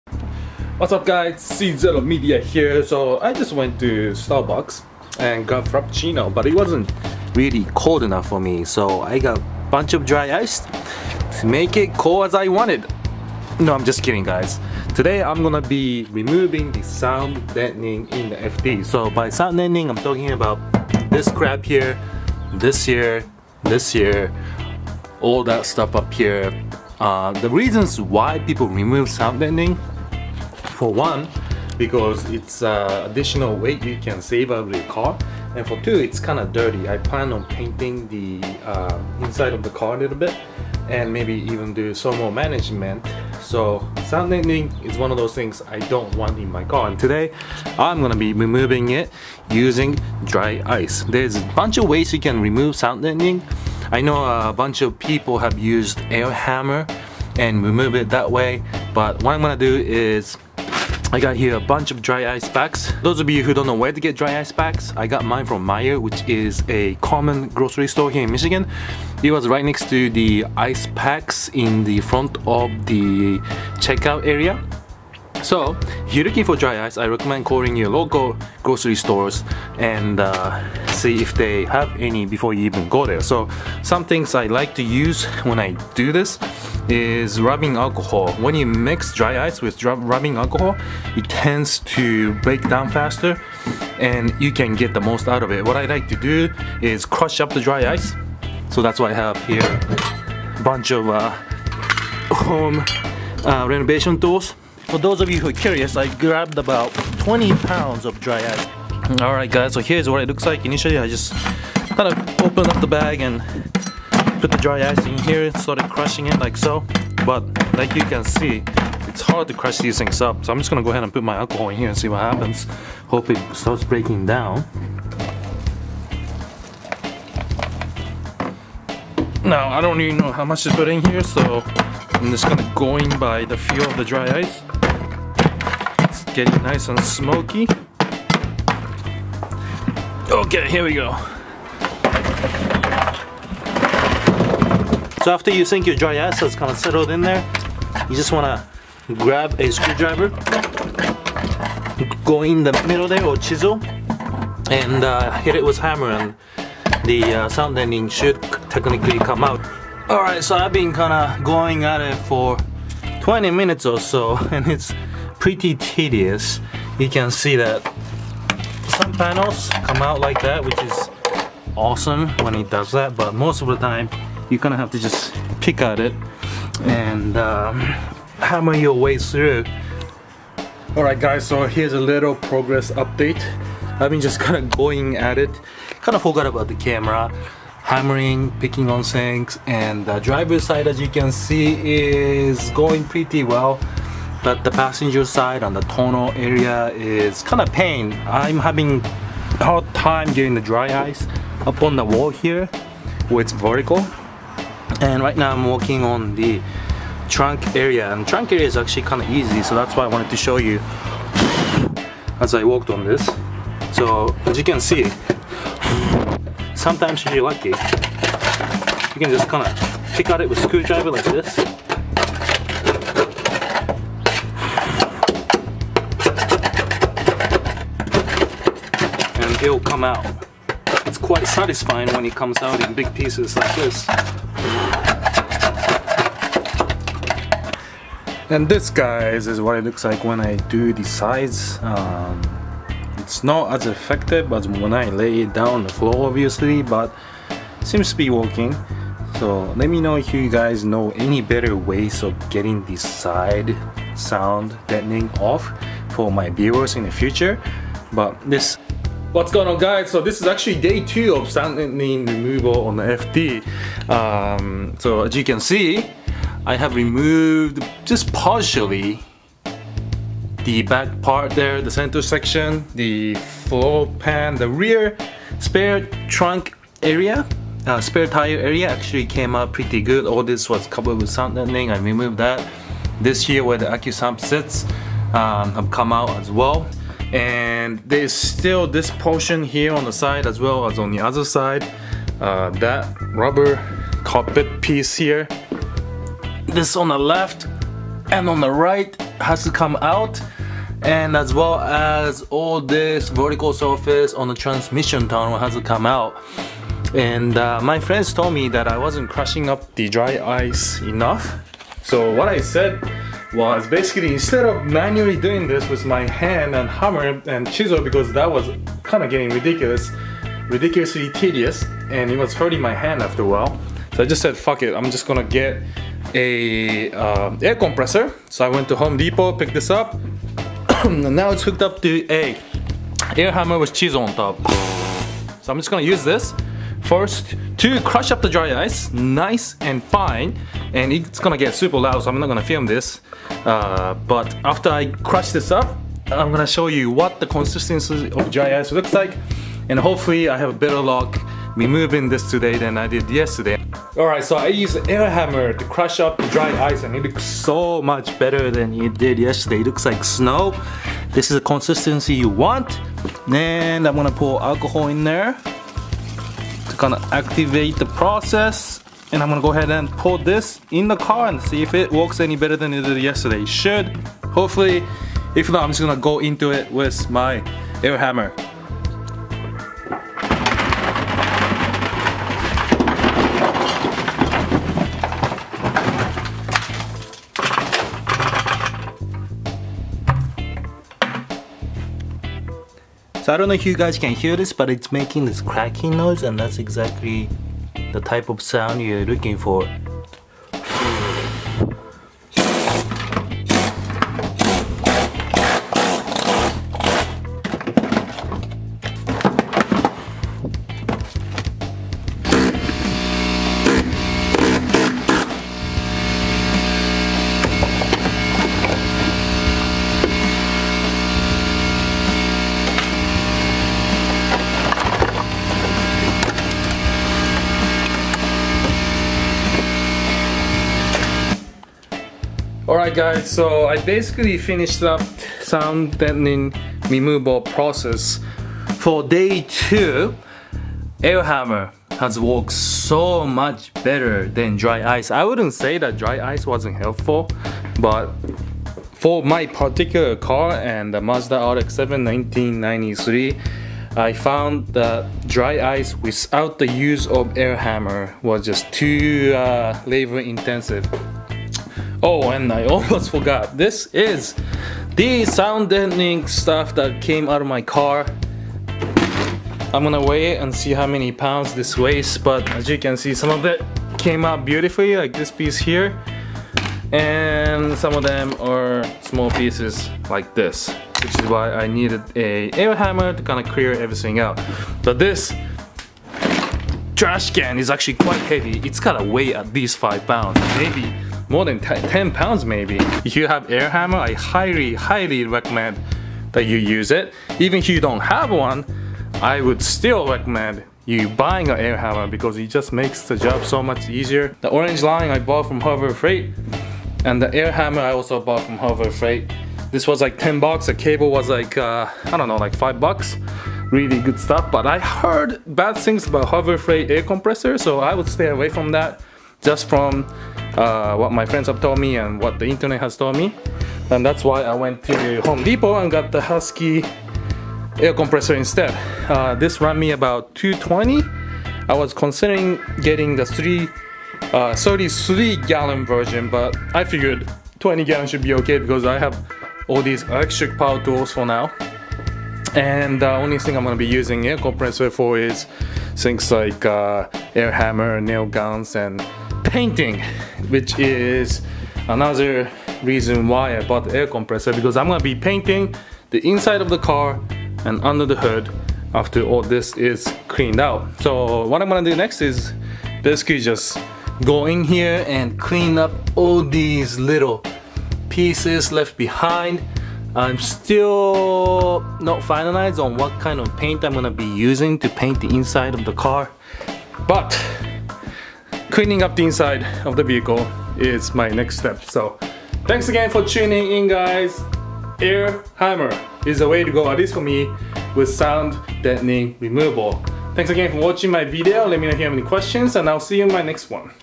How To Remove Sound Deadening - Dry Ice Method! - Wide Body V8 FD RX7 Build Video Series 14
Today on the FD build series, we try to remove the sound deadening using the famous dry ice method. However, due to some unknown technical difficulties, i decided to purchase an air hammer and scrape the sound deadening away instead. This method was much easier and faster than simply using dry ice.